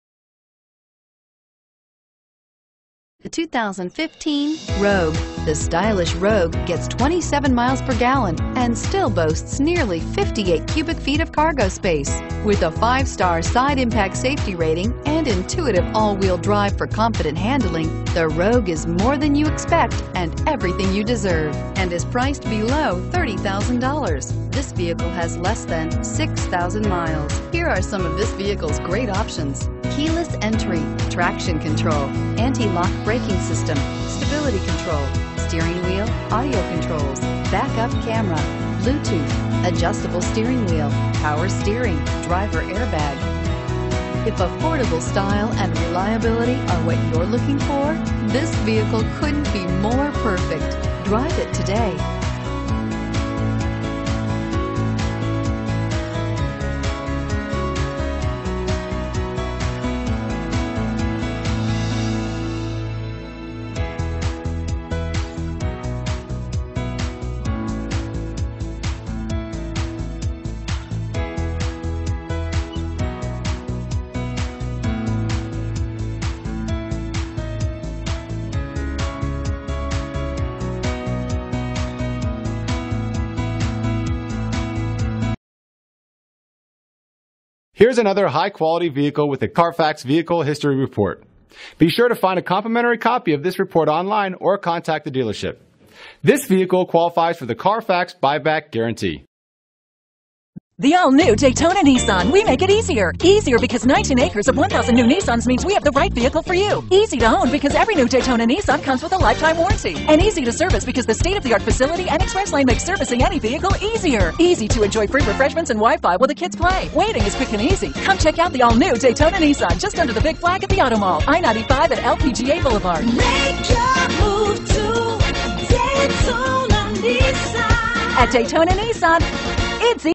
2015 Nissan Rogue Daytona Beach Florida P6458
2015 Nissan Rogue SV

Daytona Nissan
950 North Tomoka Farms Rd.

Carfax No Damage Reported. Carfax No Accidents Reported. Tire Specific Low Tire Pressure Warning, Side Impact Beams, Rear Child Safety Locks, Outboard Front Lap And Shoulder Safety Belts -inc: Rear Center 3 Point, Height Adjusters and Pretensioners, Electronic Stability Control (ESC).*Fully-Loaded with Additional Options*Wireless Streaming, Wheels: 17 Aluminum-Alloy, Variable intermittent wipers, Urethane Gear Shift Knob, Trip computer, Transmission: Xtronic CVT Automatic -inc: sport mode switch,, Tires: P225/65R17 AS, Tire Specific Low Tire Pressure Warning, Tailgate/Rear Door Lock Included w/Power Door Locks, Systems Monitor.*Visit Us Today *Stop by Daytona Nissan located at 950 North Tomoka Farms Rd, Daytona Beach, FL 32124 for a quick visit and a hassle-free deal!